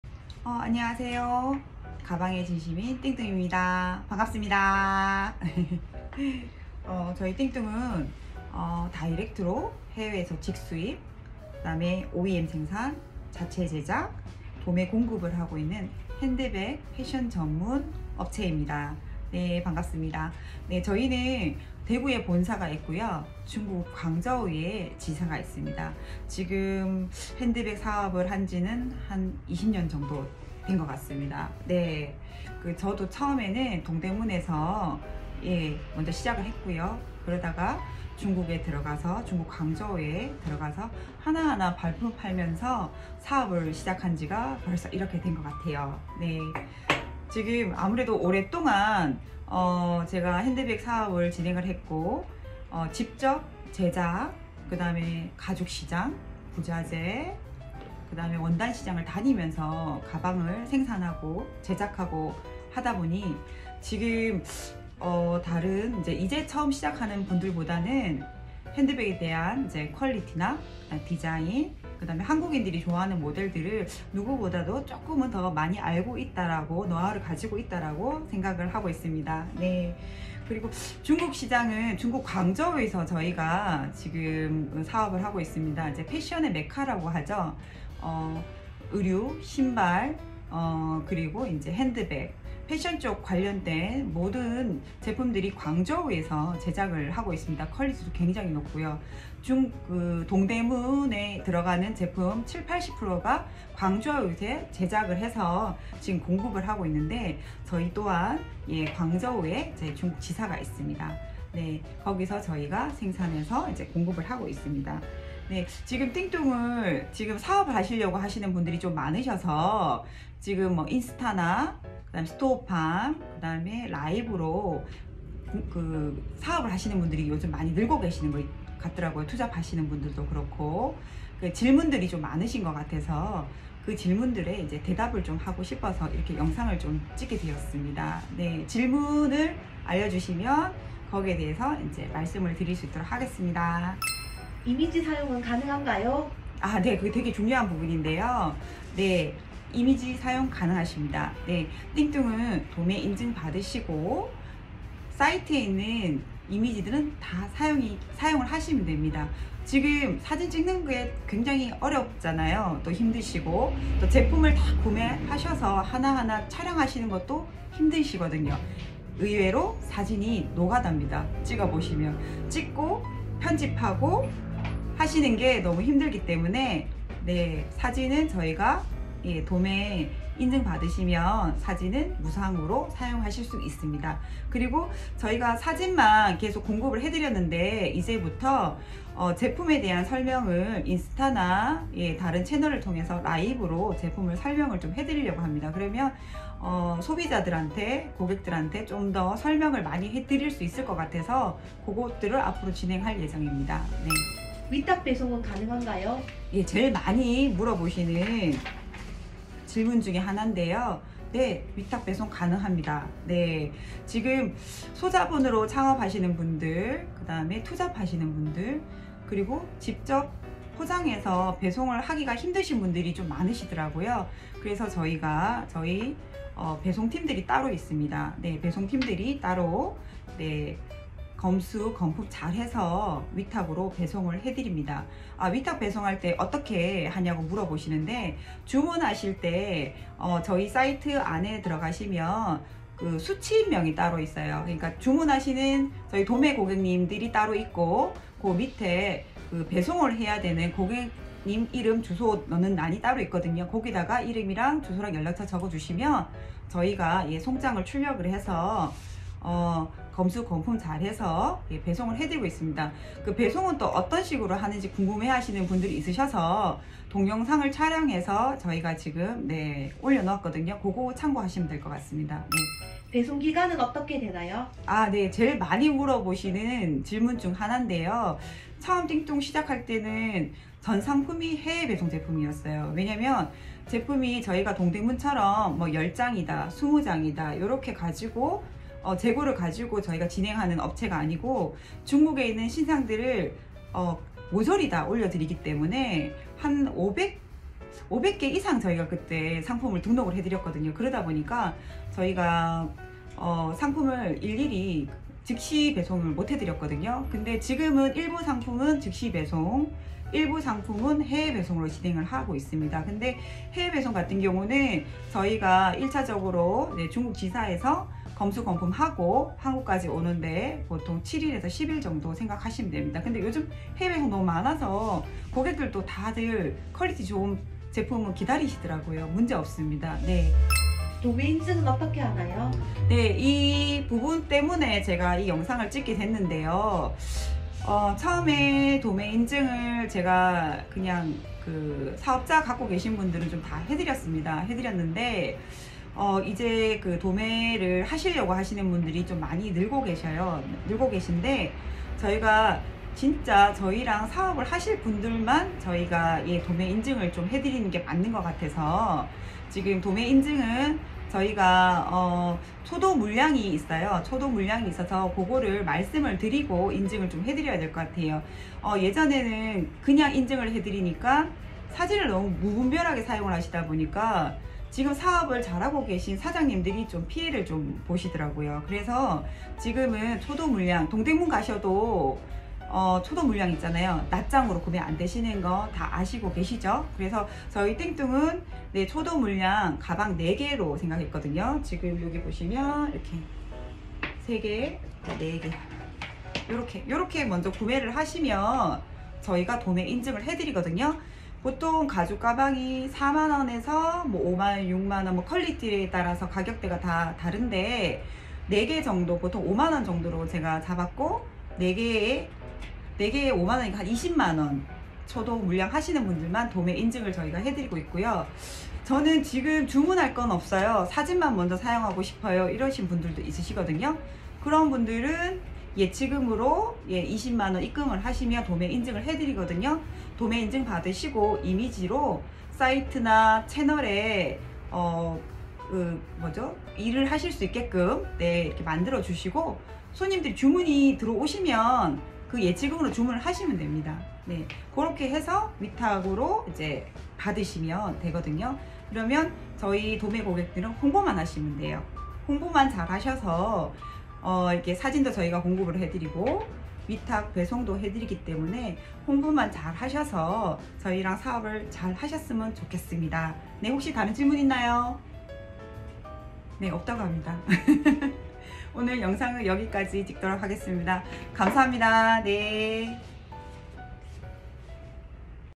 [GLM] 띵뚱마켓 l 이용 가이드 완벽 정리
📌 주문 방법

안녕하세요, 가방에 진심인 띵뚱입니다 :)

오늘은 띵뚱닷컴 도매 고객님들께서 정말 많이 여쭤보셨던 질문들을 추려 답변해 보았습니다!
이미지 무상 사용 여부부터 도매회원 인증 방법까지! 영상 하나로 모~두 해결해 드릴게요

띵뚱 도매를 사랑해 주시는 많은 고객님들께 언제나 감사하단 말씀을 드립니다.

도매 문의는 카톡 '이바이윈'